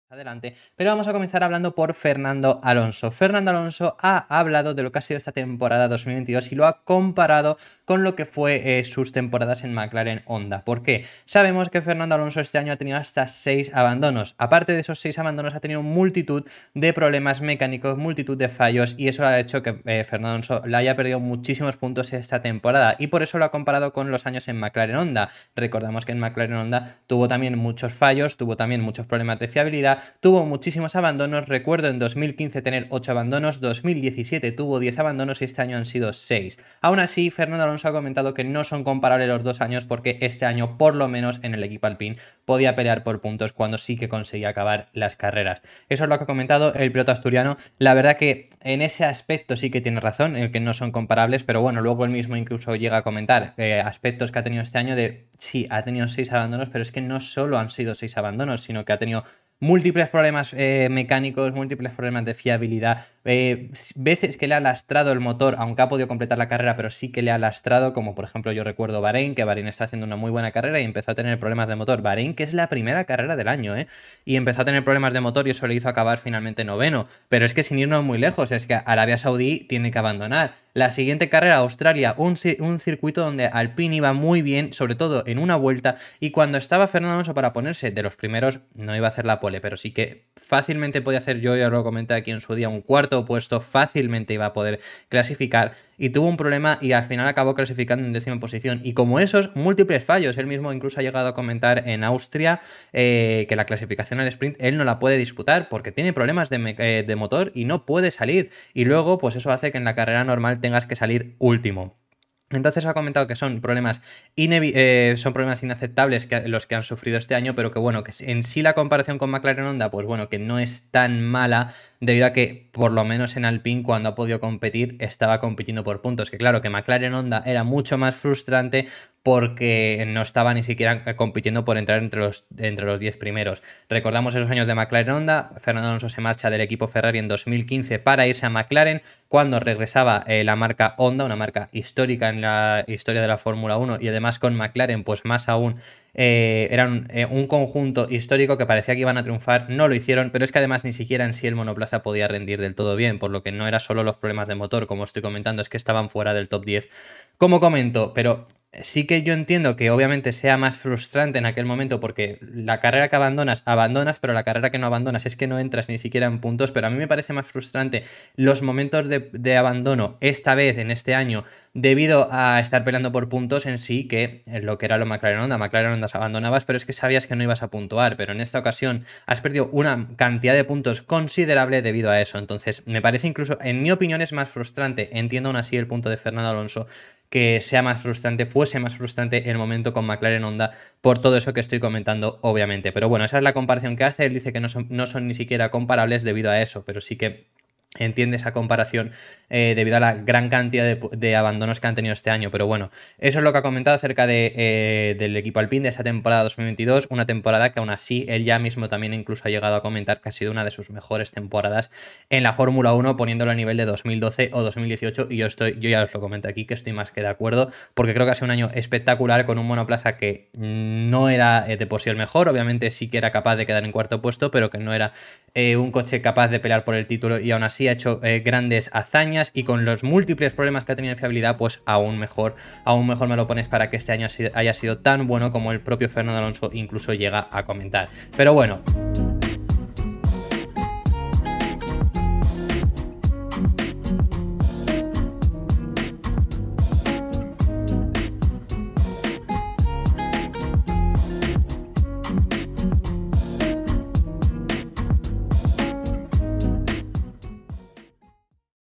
Sorprendentes DECLARACIONES de Alonso sobre la COMPARACIÓN entre ALPINE y MCLAREN-HONDA
El piloto asturiano obtuvo seis abandonos en 2022, recordando los ocho que tuvo en su etapa en McLaren-Honda en 2015 o los diez que tuvo en 2017. Por ello, es el propio piloto quien recuerda esos años y los compara con 2022.

¿Fue 2022 un año más frustrante que la etapa en McLaren-Honda?

Recordad que podéis verme y seguirme a partir de ahora en Car and Driver, donde estaré colaborando con artículos y vídeos.

Fotografías de: Mercedes, Red Bull Media, Ferrari Media, McLaren, Alpine, Aston Martin, AlphaTauri, Haas, Williams, Alfa Romeo.